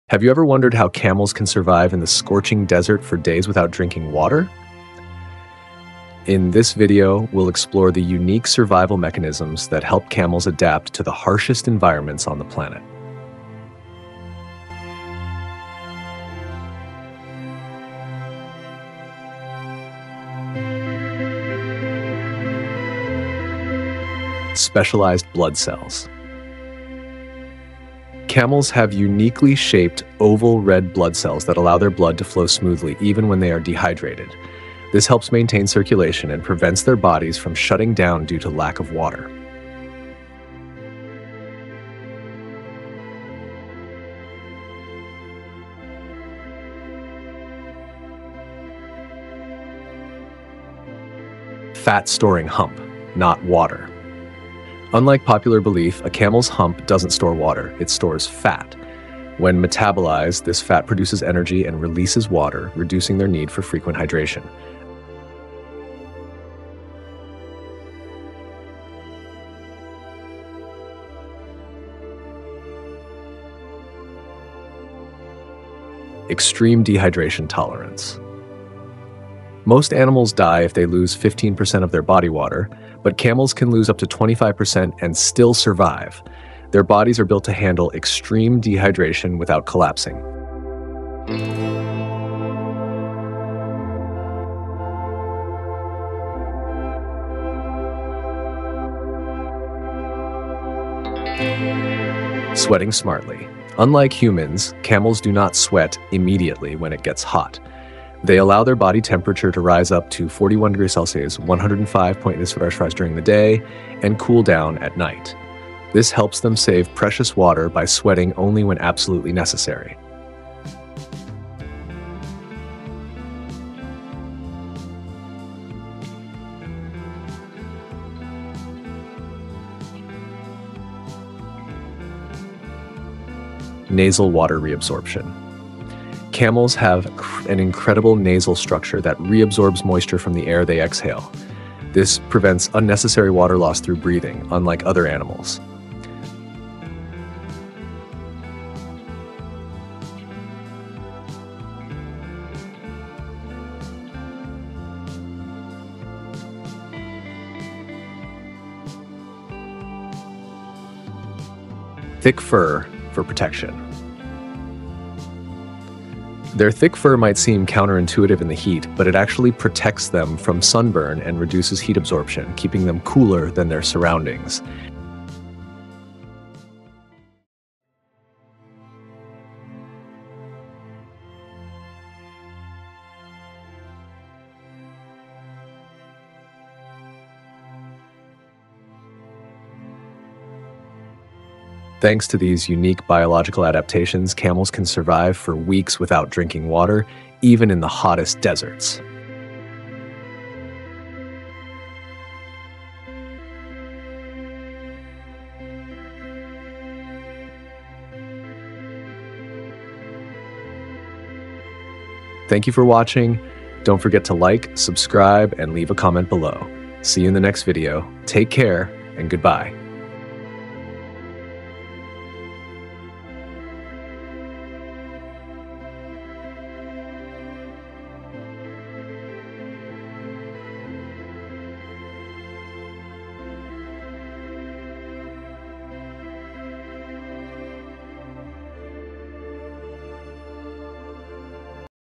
Decoding the Extraordinary Heat Resistance of Camels
🐪 How do camels survive in extreme desert heat without sweating excessively? What makes them capable of enduring temperatures up to 50°C (122°F) without suffering from dehydration? In this video, we decode the incredible heat resistance of camels and explore the fascinating adaptations that make them true masters of desert survival! 🏜️🔥

🔎 What You’ll Discover in This Video:
✅ Why camels don’t sweat immediately, even in extreme heat
✅ The secret behind their oval-shaped red blood cells 🩸
✅ How their nostrils help them conserve water while breathing 👃
✅ The surprising function of their hump (hint: it’s not for water storage!) 🐫
✅ Why camels’ thick fur actually helps them stay cool 🧥
✅ How camels can drink up to 40 gallons of water in minutes without harm! 💦

💡 Camels are among the most heat-resistant creatures on Earth, thanks to their unique biological adaptations. Scientists continue to study their incredible survival mechanisms to apply them in fields like human medicine and climate resilience!

👉 Watch now to unlock the secrets of nature’s ultimate desert survivor! 🎥